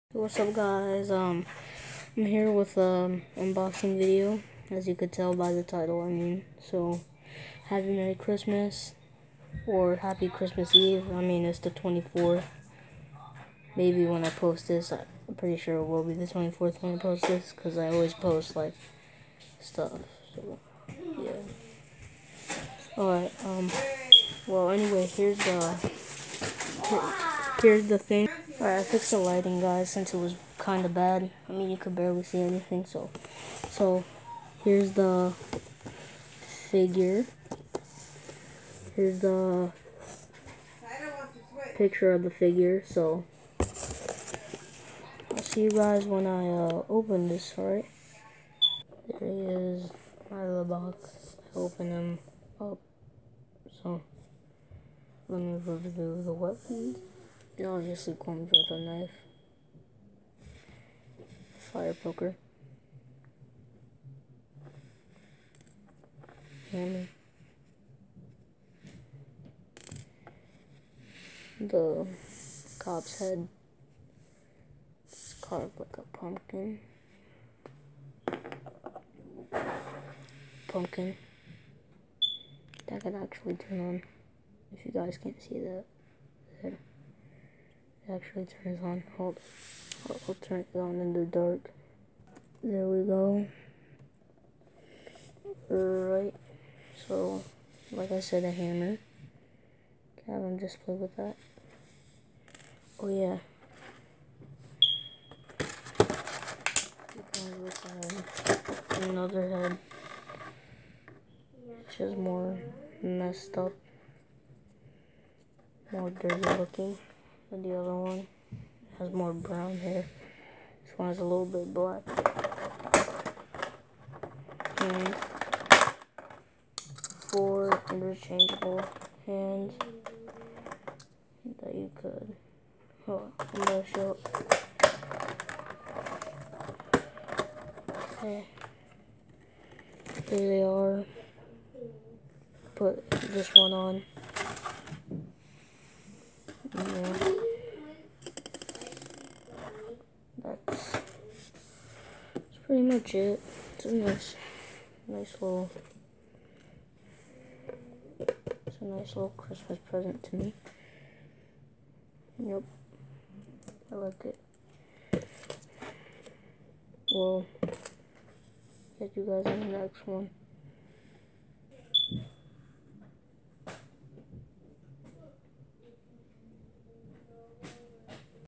My 2018 Michael Myers action figure review
Don't mind why I did that hand thing at the beginning it's a habit.